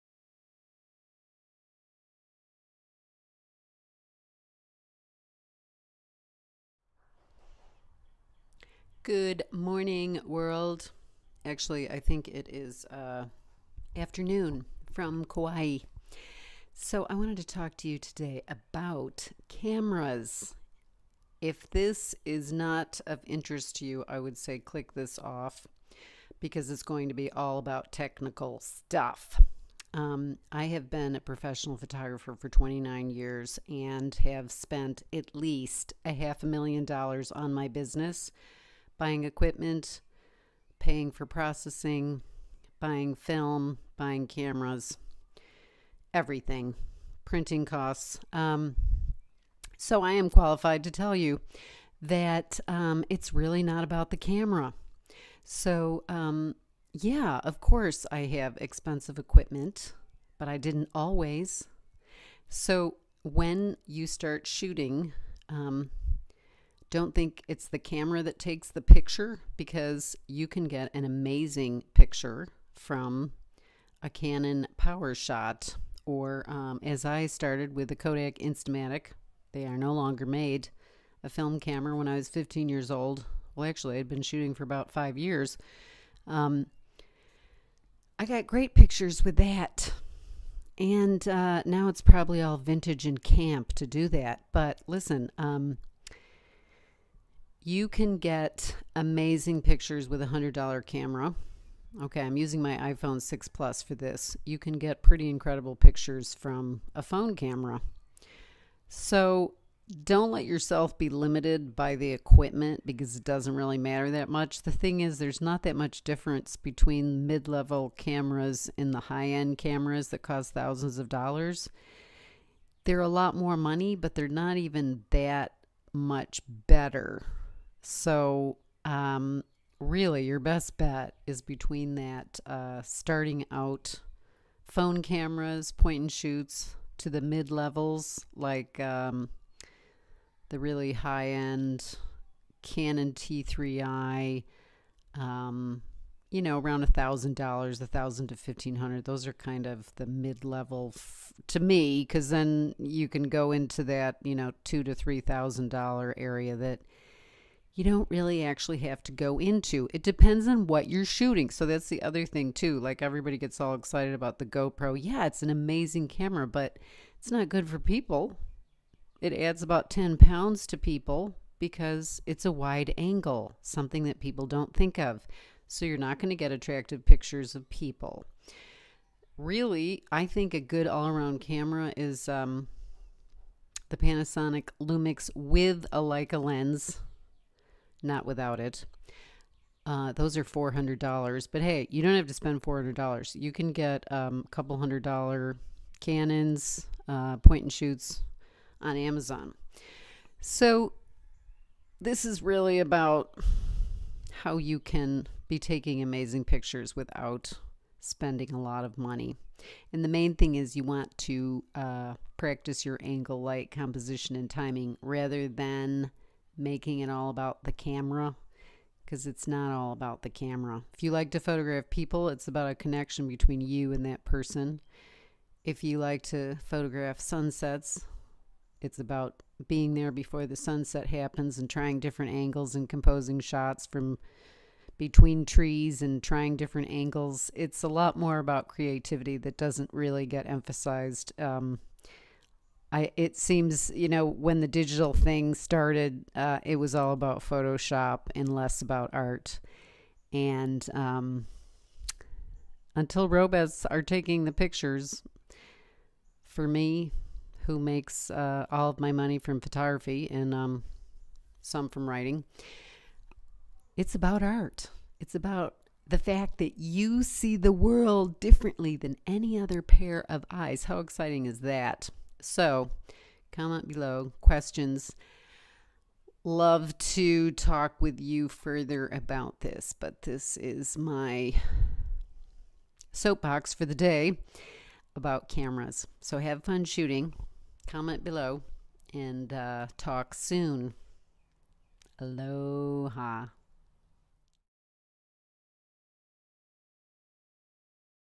What Cameras I Use and Why
It's not about your camera or pixels, it's about your vision.

Don't use lack of money to buy pricey cameras as your excuse - you can take amazing photos with any camera.

You'll learn that you can shoot with a phone, point and shoot or other low end camera and still get great photos.cameras

Books on Amazon

course ID 103590

New Issue of my magazine, Vagabond Travel Photography Mag
Living in Hawaii, is the new issue